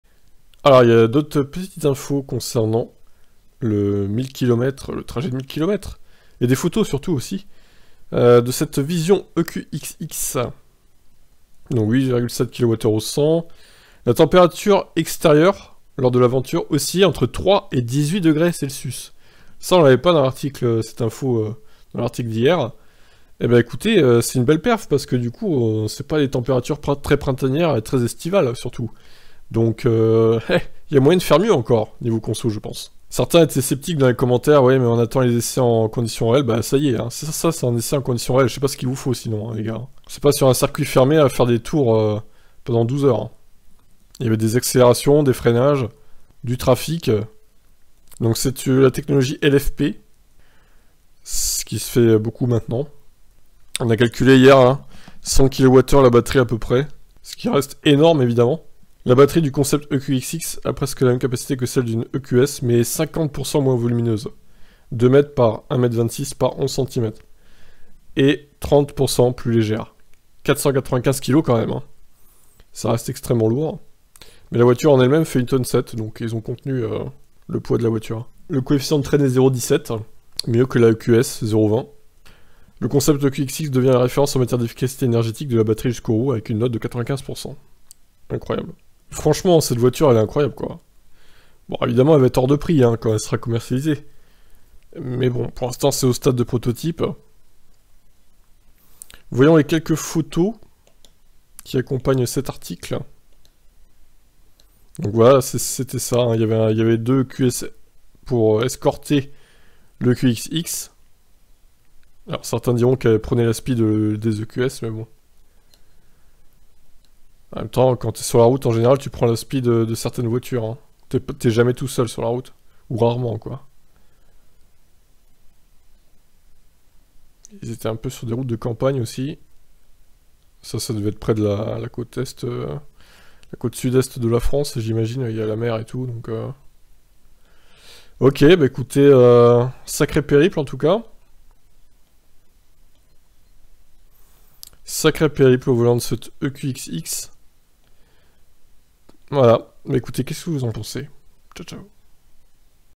Des photos et de nouvelles infos concernant le trajet record de 1000 km de la Mercedes EQXX
La météo était loin d'être clémente !